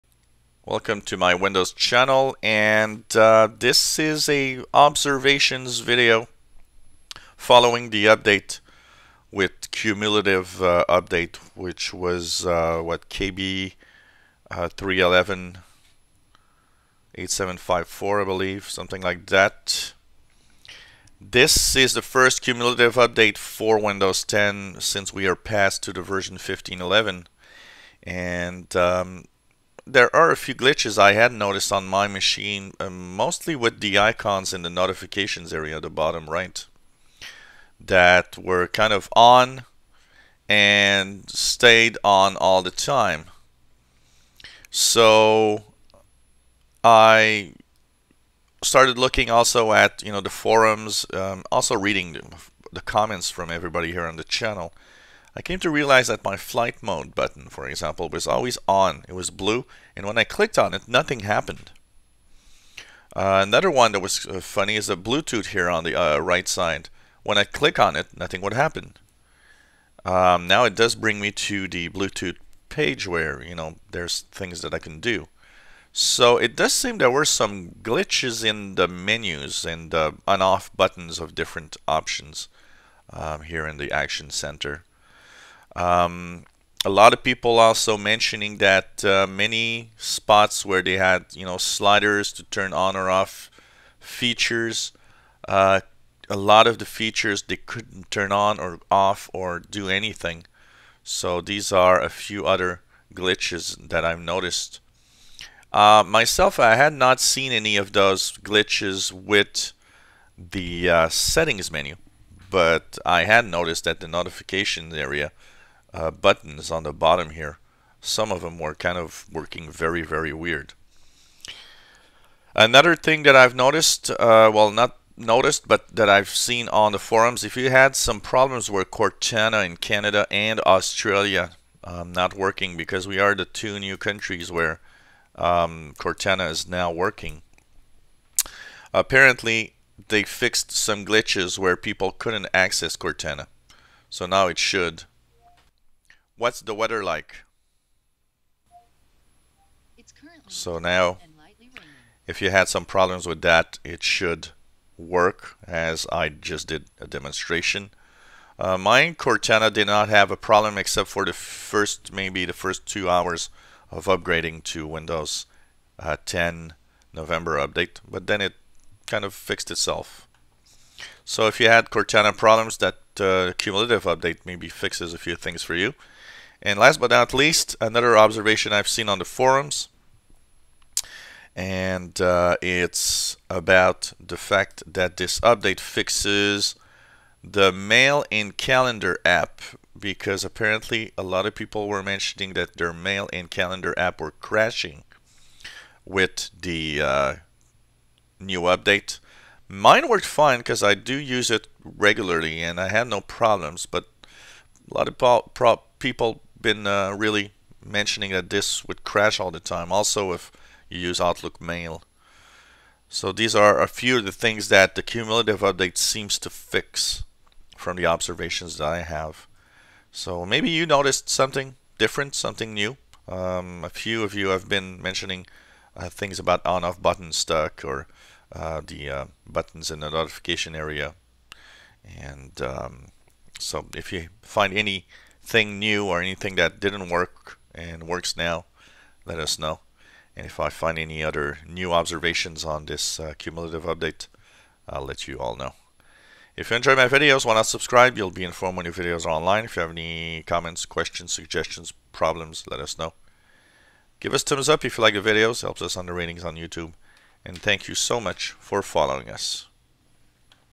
Windows 10 Observations after Cumulative update KB3118754 November 19th 2015
Observations of the new cumulative update for Windows 10 version 1511, fixes are seen in the action center buttons, some on off sliders, Cortana fixes and also mail and calendar crashes fix in here